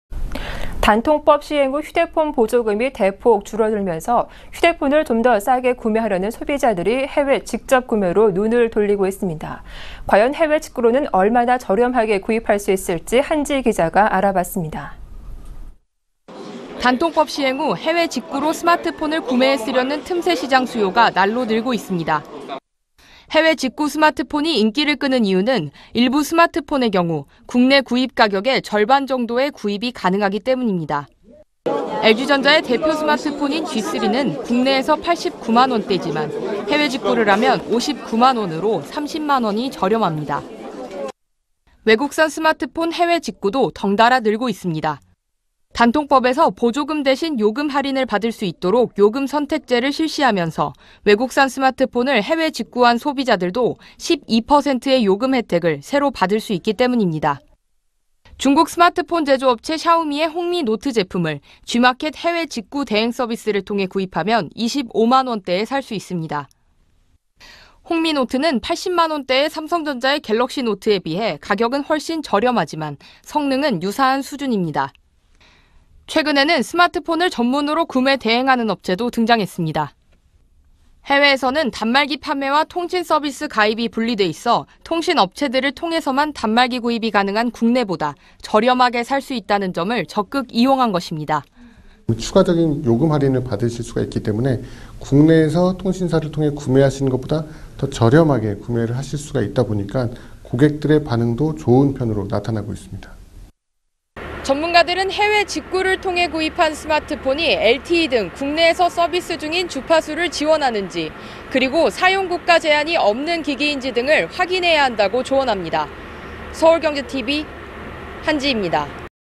단통법이 바꿔놓은 풍속도 스마트폰 직구 ‘급증’
[앵커]
단말기유통구조개선법 시행 후 휴대폰 보조금이 대폭 줄어들면서 휴대폰을 좀 더 싸게 구매하려는 소비자들이 해외 직접구매로 눈을 돌리고 있습니다. 해외직구로 휴대폰을 얼마나 저렴하게 살 수 있는지 한지이 기자가 알아봤습니다.

[기자]
단통법 시행 후 해외 직구로 스마트폰을 구매해 쓰려는 틈새시장 수요가 날로 늘고 있습니다. 해외 직구 스마트폰이 인기를 끄는 이유는 일부 스마트폰의 경우 국내 구입 가격의 절반 정도에 구입이 가능하기 때문입니다. LG전자의 대표 스마트폰인 G3는 국내에서 89만원 대이지만 해외 직구를 하면 59만원으로 30만원이 저렴합니다.
외국산 스마트폰 해외 직구도 덩달아 늘고 있습니다. 단통법에서 보조금 대신 요금할인을 받을 수 있도록 ‘요금선택제’를 실시하면서 외국산 스마트폰을 해외 직구한 소비자들도 12%의 요금할인혜택을 새로 받을 수 있기 때문입니다. 중국 스마트폰 제조업체 샤오미의 ‘홍미노트’제품을 G마켓 해외직구 대행 서비스를 통해 구입하면 25만원대에 살 수 있습니다. 홍미노트는 80만원대의 삼성전자의 갤럭시 노트에 비해 가격은 훨씬 저렴하지만 성능은 유사한 수준입니다.
최근에는 스마트폰을 전문으로 구매 대행하는 업체도 등장했습니다. 해외에서는 단말기 판매와 통신 서비스 가입이 분리돼 있어 통신업체들을 통해서만 단말기 구입이 가능한 국내보다 저렴하게 살 수 있다는 점을 적극 이용한 것입니다.

[인터뷰] 박종일 대표 / 착한텔레콤
(해외에서 단말기를 구입하시는 분들은) 추가적인 요금 할인을 받으실 수 있기 때문에 국내에서 통신사를 통해 구매하시는 것보다 더 저렴하게 구매하실 수 있다 보니까 고객들의 반응도 좋은 편으로 나타나고 있습니다.

전문가들은 해외직구를 통해 구입한 스마트폰이 LTE 등 국내에서 서비스 중인 주파수를 지원하는지, 사용국가제한이 없는 기기인지 먼저 확인해야 한다고 조언합니다.

서울경제TV 한지이 입니다.